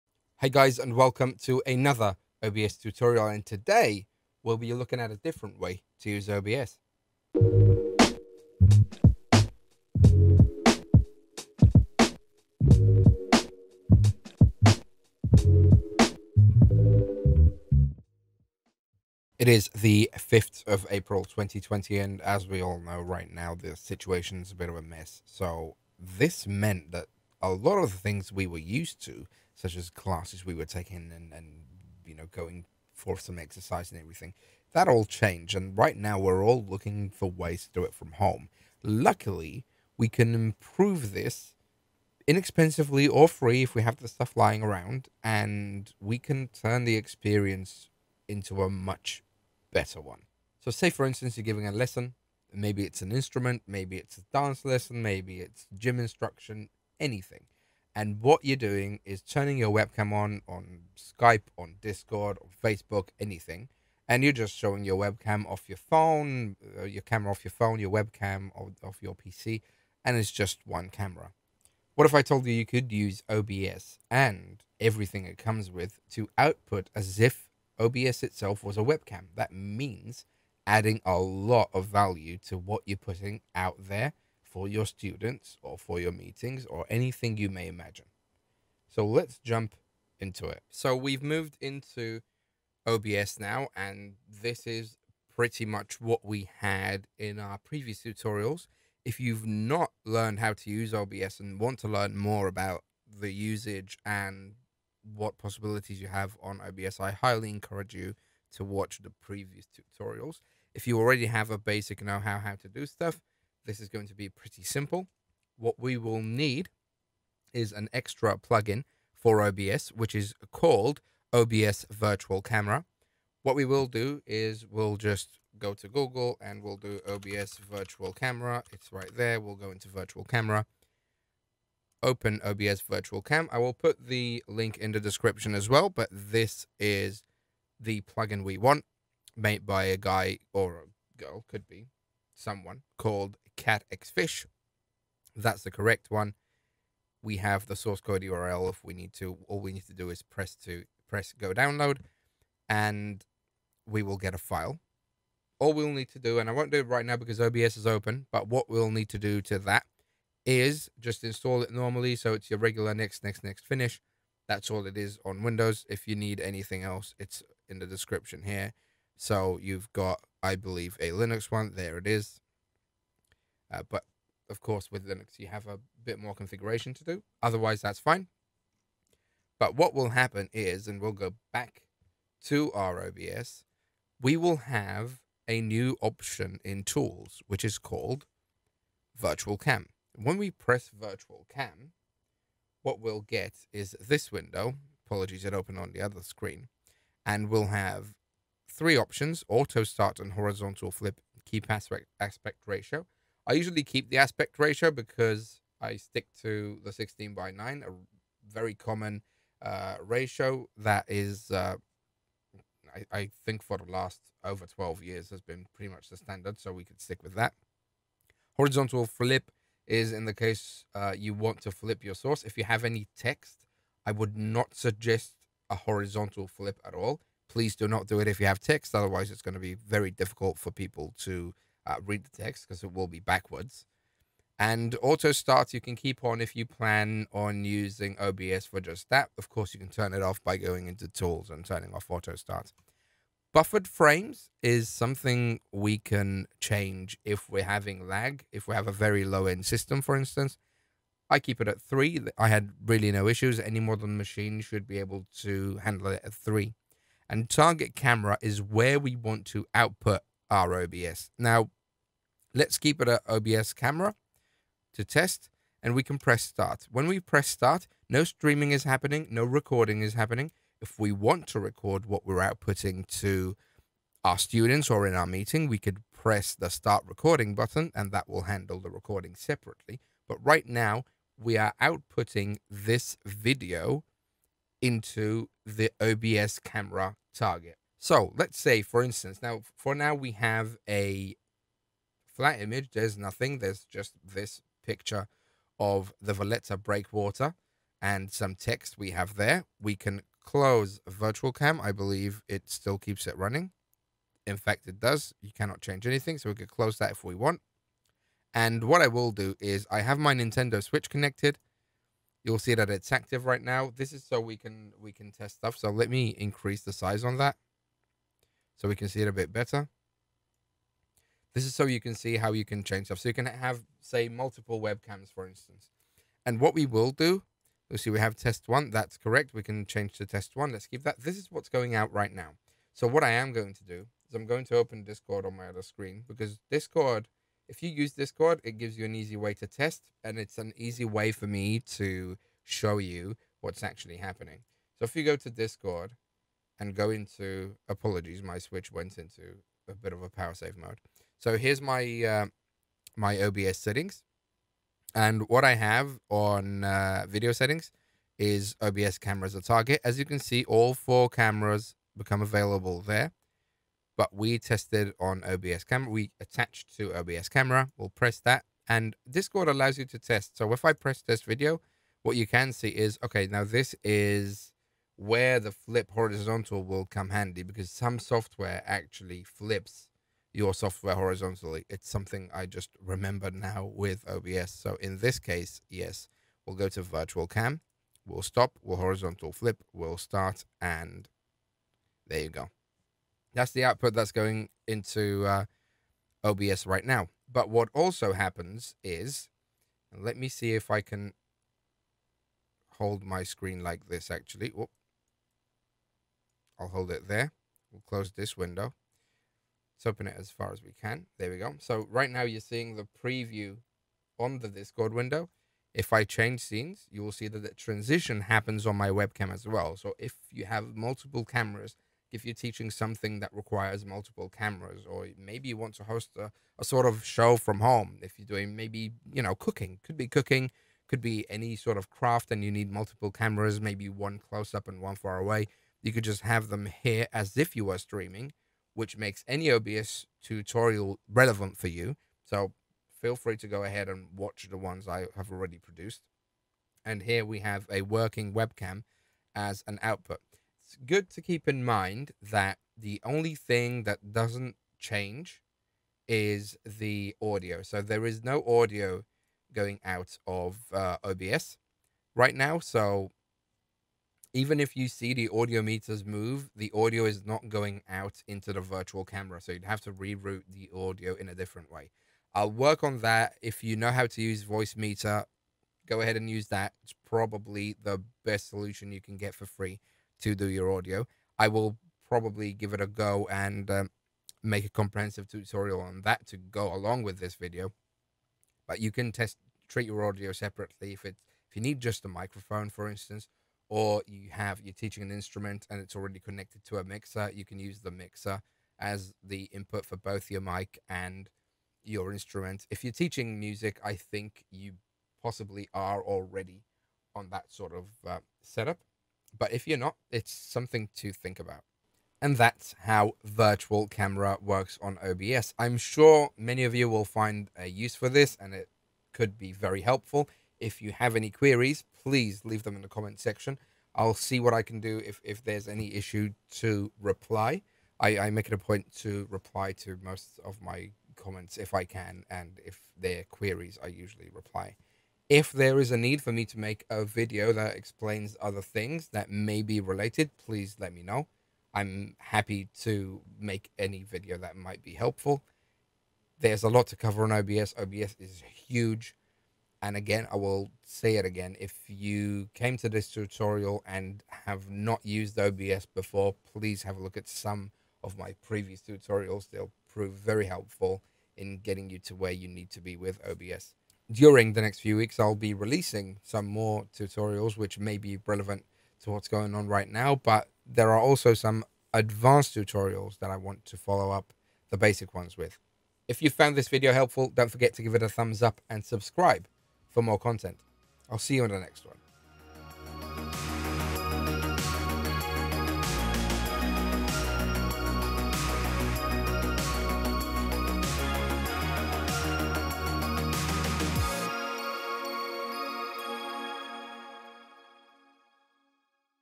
How to use OBS as a webcam on Discord, Microsoft Teams, Zoom, Skype and more!
Have you ever wanted to have better control over your livestreams, conferencing or online lessons? It's time to leverage OBS's capability to give you the edge.

I stream from home but I'm also a producer for esports tournaments here on the lovely island of Malta in which I apply many practices which can also be used at home.